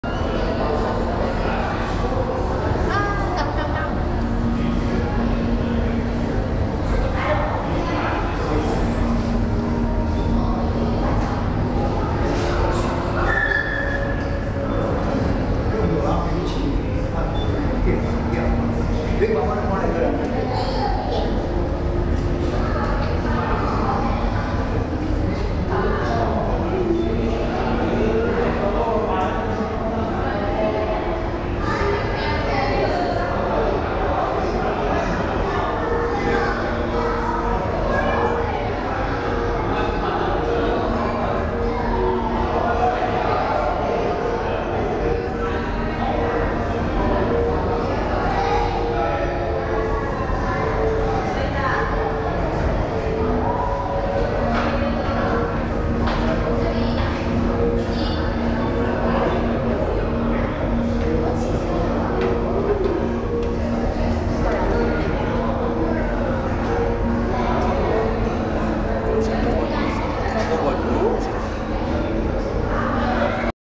Hong Kong Ocean Park - Grand Aquarium Sideport View
The view from a side port of the Grand Aquarium in Hong Kong's Ocean Park